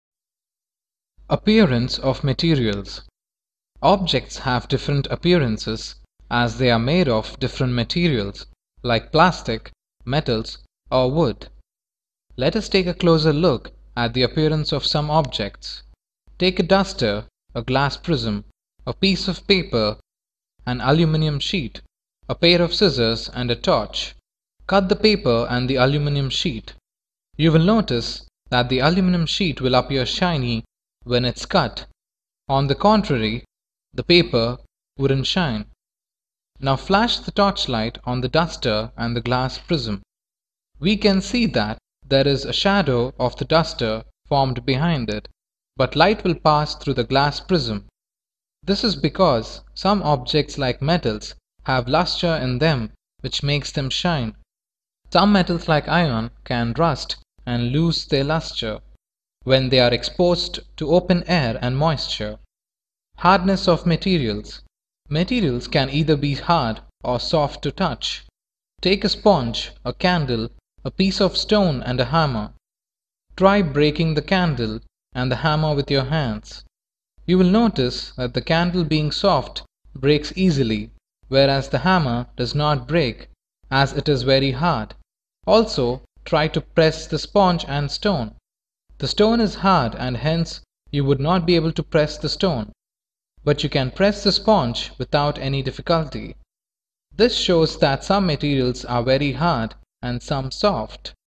Materials and their Appearance - Science
APPEARANCE OF MATERIALS

Objects have different appearance as they are made of different materials like plastic, metals or wood.
Let us check the appearance of the objects.
Take a duster, a glass prism, a piece of paper, aluminum sheet, a pair of scissors and a torch.
Cut the paper and aluminum sheet.
You will notice that the aluminum sheet will appear shiny when it is cut.
Now, flash the torch light on the duster and the glass prism.
We can see that the duster will not make any difference but the glass prism will shine when light is passed through it.
This is because some objects like metals have lustre in them which makes them shine.
Some metals like iron can rust and lose their lustre when it is exposed in open air and moisture.

HARDNESS OF MATERIALS

Materials can be either hard or soft to touch.
Take a sponge, a candle, a piece of stone and a hammer.
Try breaking the candle and the hammer with your hands.
You will notice that the candle being soft breaks easily whereas the hammer does not break as it is very hard.
Also, try to press the sponge and stone.
The stone is hard and you will not be able to press the stone.
But, you can press the sponge without any difficulty.
This tells us the materials can be sometimes very hard and sometimes soft.
Some materials very hard some materials soft